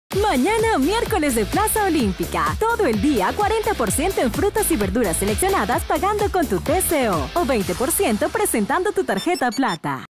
Descuentos 20 de junio
¡Mañana es Miércoles de Plaza! Frutas y verduras referencias seleccionadas con el 40%dcto pagando con TCO ó 20%dcto presentando tu Tarjeta Plata. Válido 20 de junio *AC